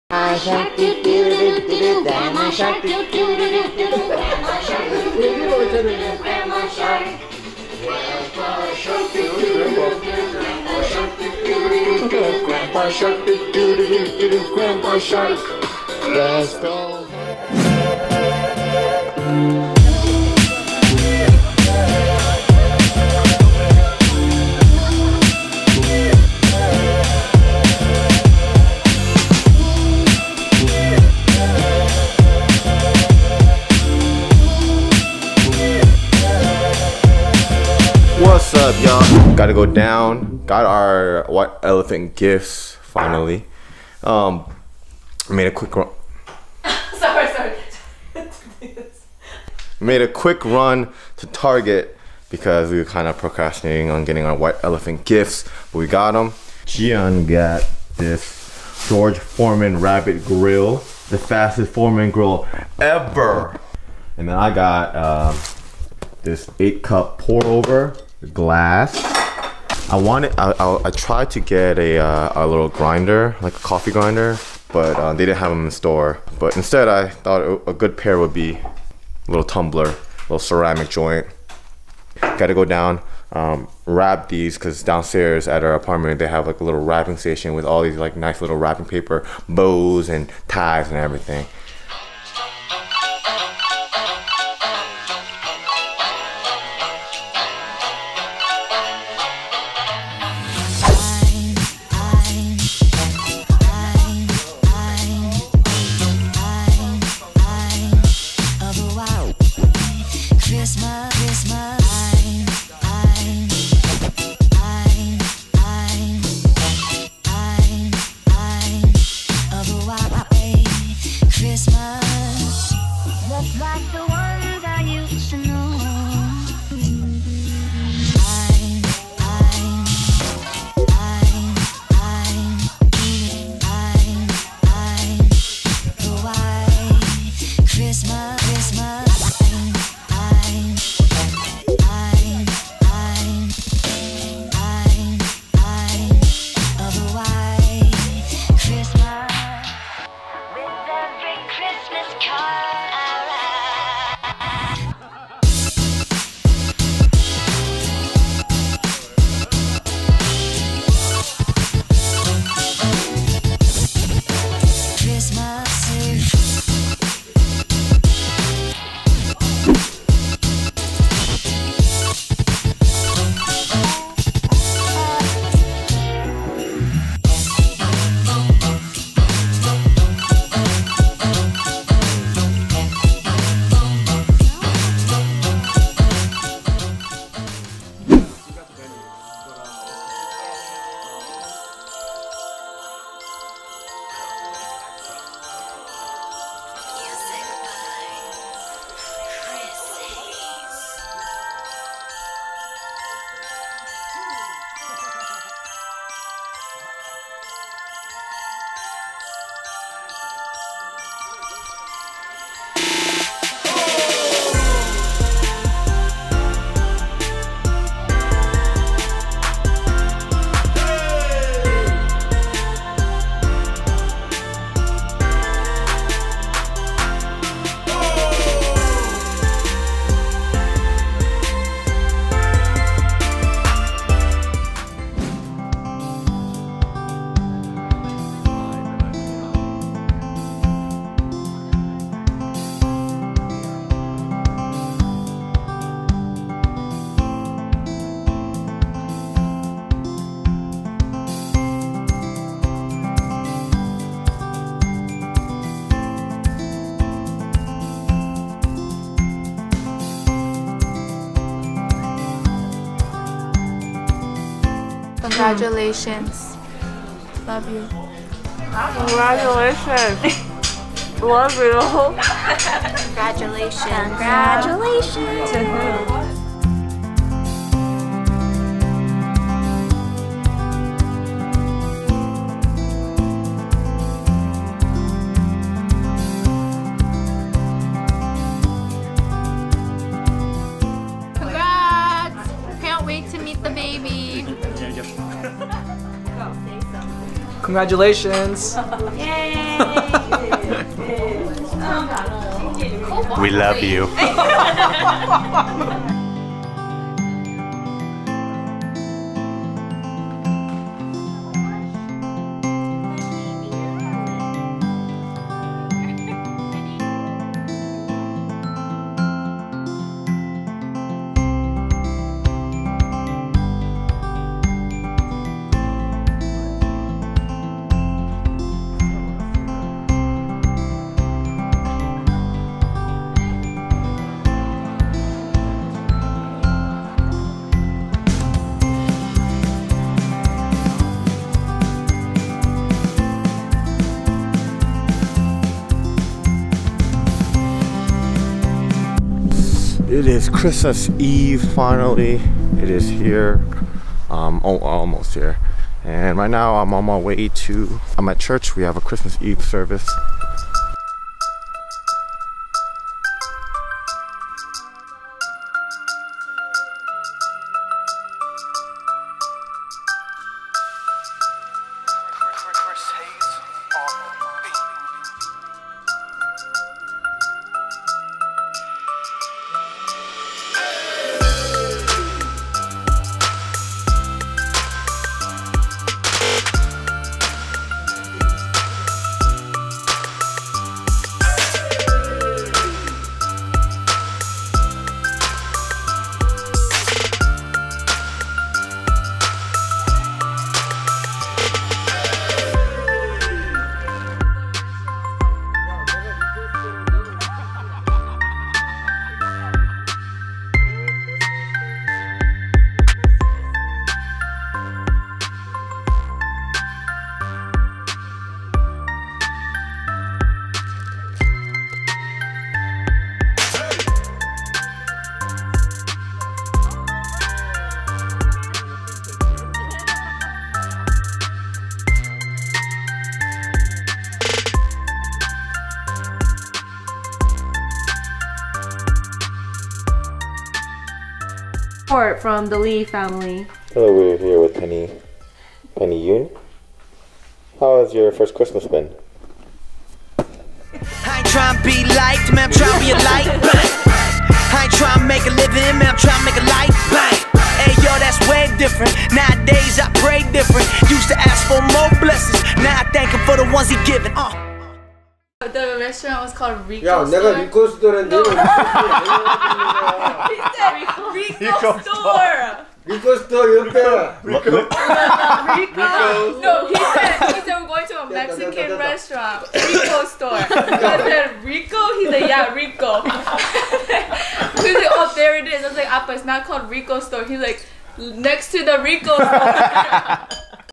STV S3E24: The Last Bits of 2018
In this episode:
- Lunch with AKweon & Jonathan and Sophia & Danny at Hangari
- Championship Victory
- Christmas Party at the Chung's
- Penny
- Christmas Party at the Kim's
- Baby Shower for Chris and Bora's baby (Evalyn)
- Dinner with Damon at Pike Kitchen
- Christmas Eve service at church
- Christmas Party with cousins at the Yoon's
- Christmas dinner with family